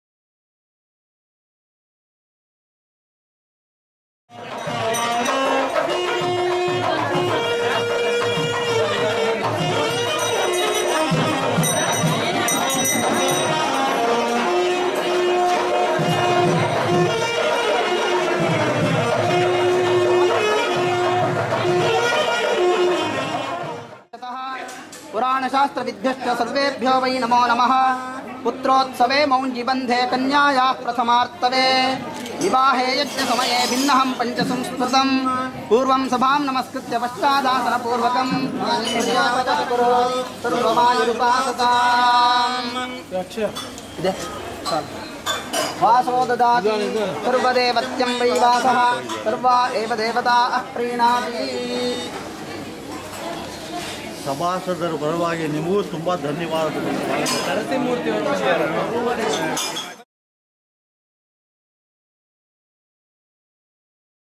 Marriage Rituals of Kannada Brahmins, Karnataka
The rituals being performed during a marriage ceremony held at Karnataka, India.